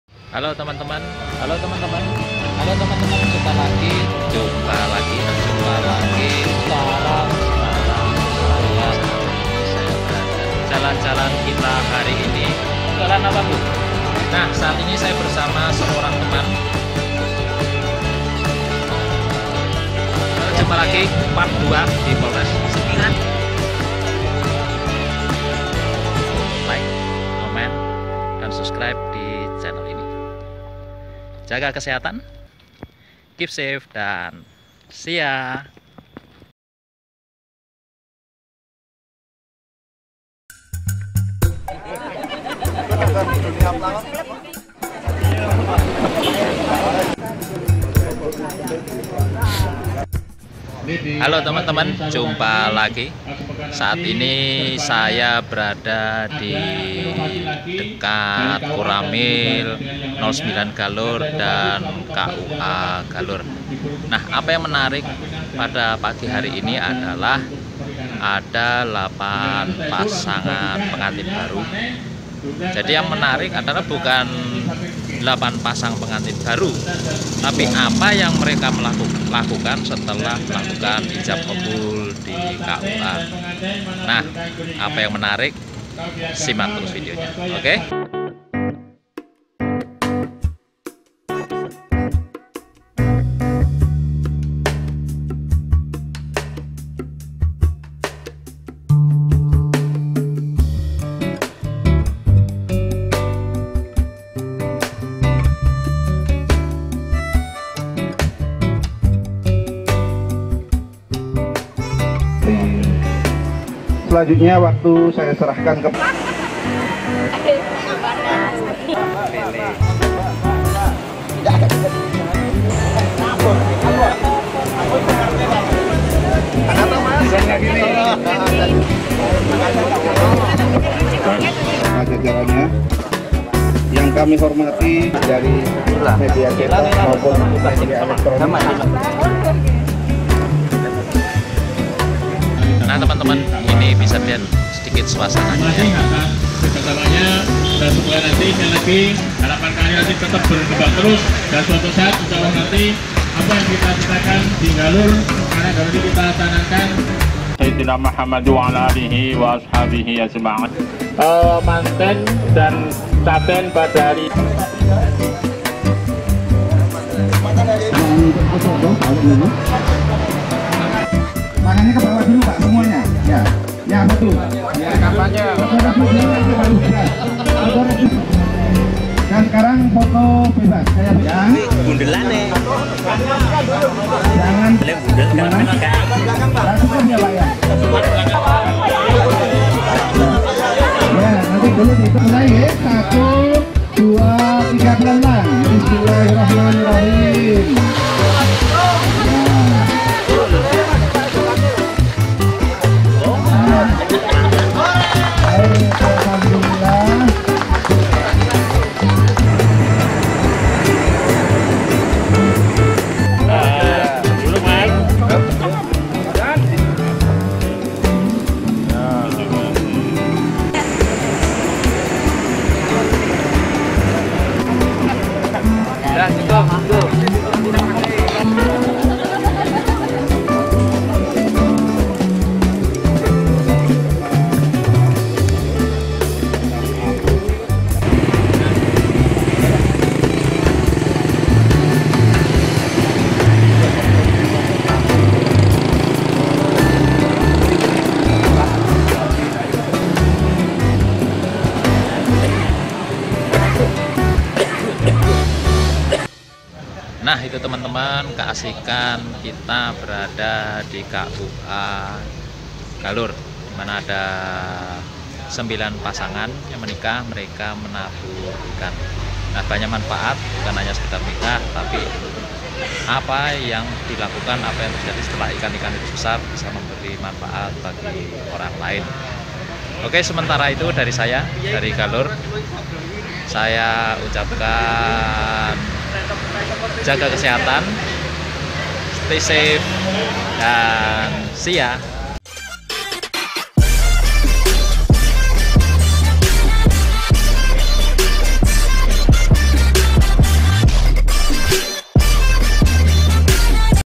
Nikah Unik di KUA Galur Tebar Ikan dan Tanam Pohon
Galur - Setelah melakukan akad nikah di KUA Galur, 9 pasang pengantin baru di Kecamatan Galur melepasliarkan berbagai jenis ikan ke sungai, Jumat (1/2). Ini merupakan tradisi baru yang tengah digencarkan kantor Urusan Agama (KUA) Galur bersama pemerintah kecamatan dan Koramil setempat kepada para pengantin baru di wilayahnya.

Tebar benih ikan dan penanaman bibit pohon dilakukan di sekitar Sungai Klampok tak jauh dari lokasi KUA Kecamatan Galur. Program yang sudah berjalan setahun ini dapat membawa misi pelestarian lingkungan serta pemberdayaan ekonomi masyarakat setempat.

Lokasi:
Sekitar KUA dan Koramil 9 Kecamatan Galur, Kabupaten Kulon Progo, DIY

(video diambil tanggal 1 Februari 2019)